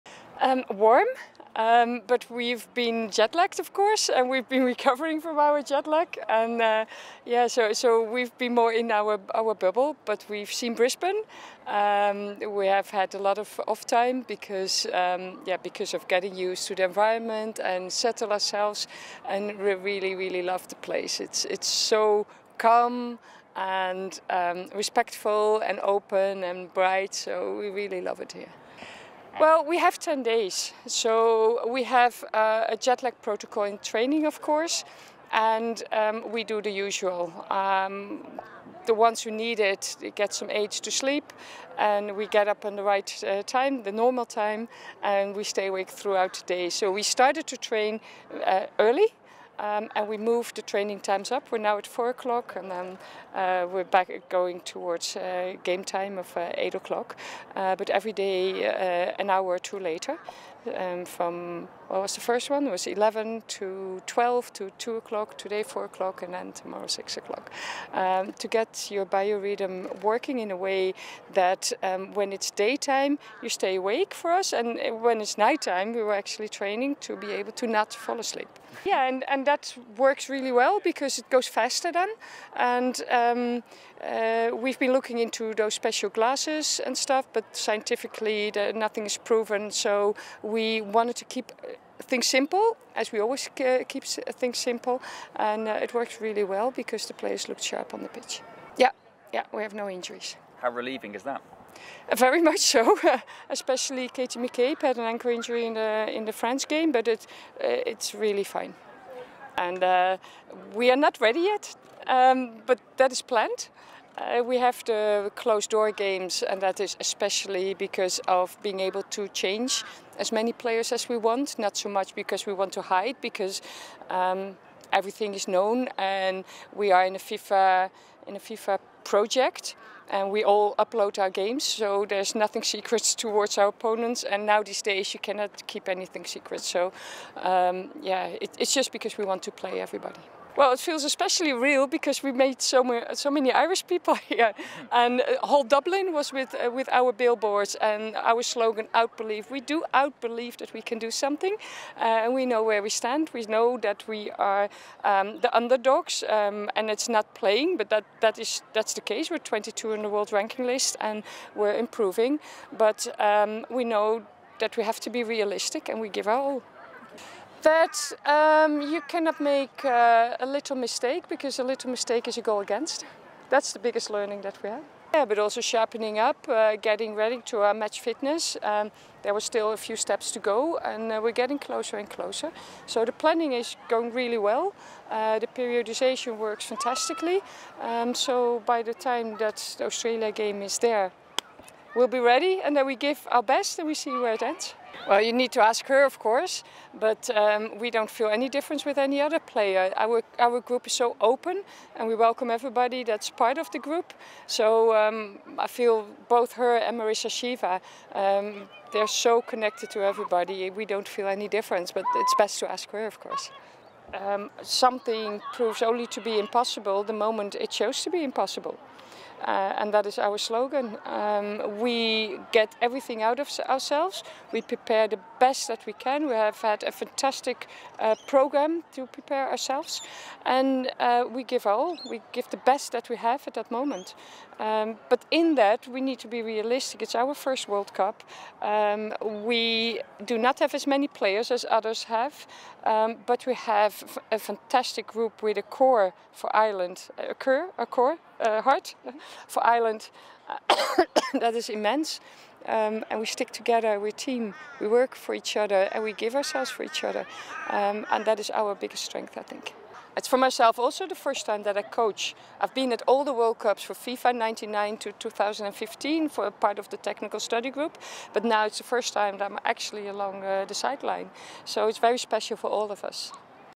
'We know we're the underdogs' Ireland manager Vera Pauw speaks ahead of World Cup opener in Sydney
Republic of Ireland manager Vera Pauw previews her side's chances in the upcoming World Cup finals as they prepare to take on hosts Australia in the opening match.

Ireland's World Cup preparations were boosted on Wednesday when FIFA gave the Girls in Green a training ground upgrade.

They were due to train at Goodwin Park in Brisbane, home of semi-professional side Olympic FC.

However, when FIFA inspected the playing surface, they decided to move Vera Pauw’s side to Meakin Park, a half-hour journey from their plush Emporium Hotel base in the city centre.

They were initially moved to The Underwood, and used that facility earlier this week.

Have you downloaded the Irish Mirror app?

- Search for Irish Mirror in the App Store if you're on iOs

GET IN TOUCH WITH THE IRISH MIRROR